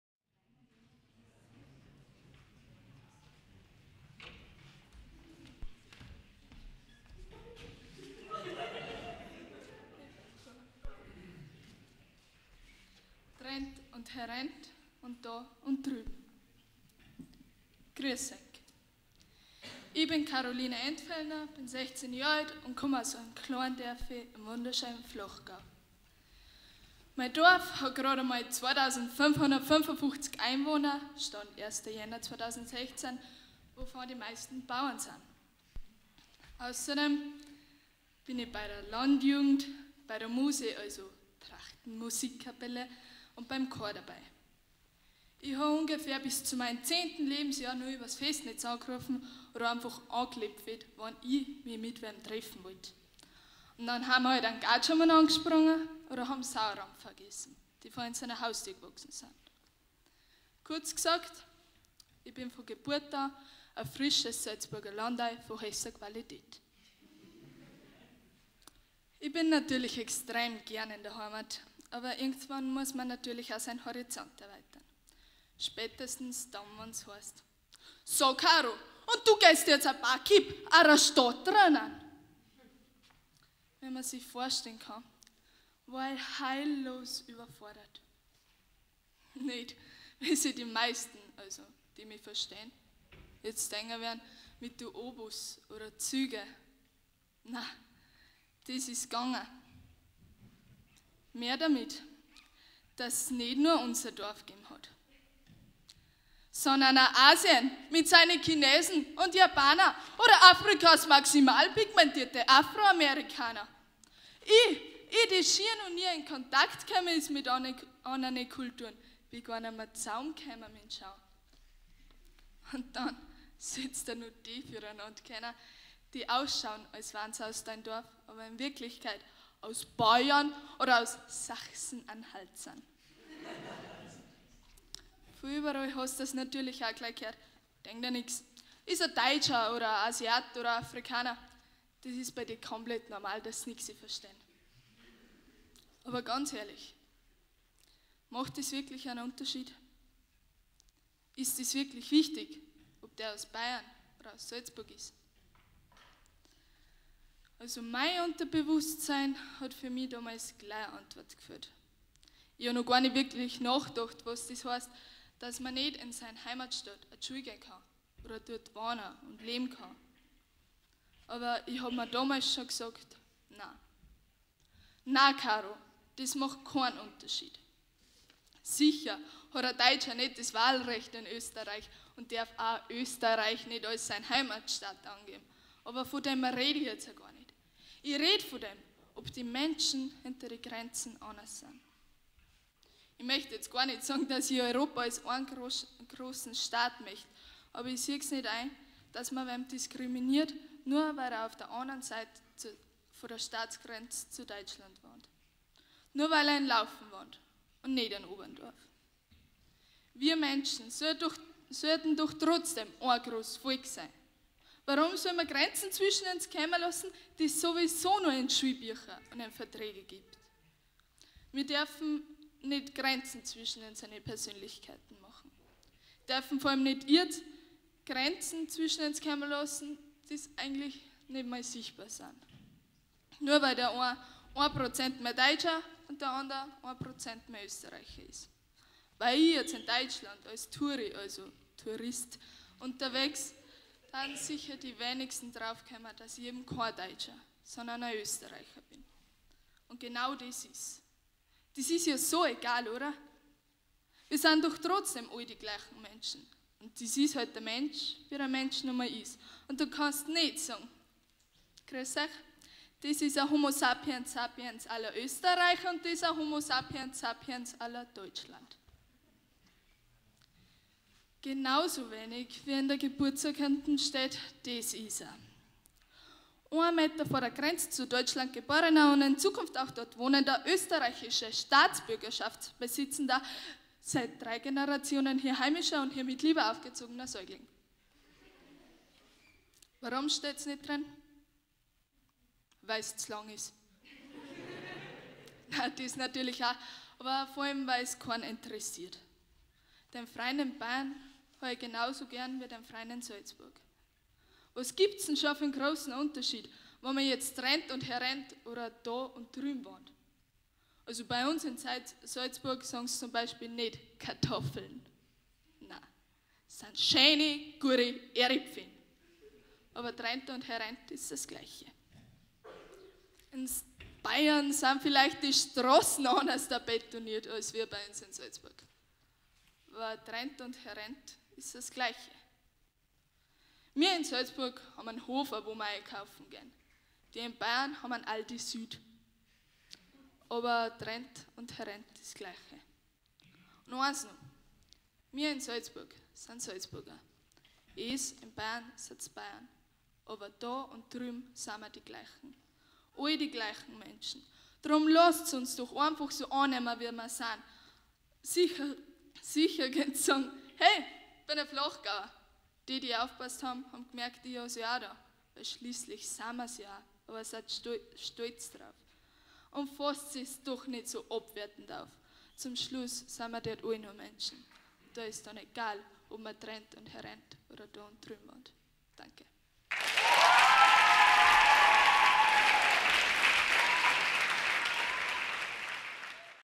Karoline Entfellner | Jugendredewettbewerb 2016 | FS1
Karoline Entfellner vom Jugendredewettbewerb 2016 mit "Drent und herent oder do und drübn".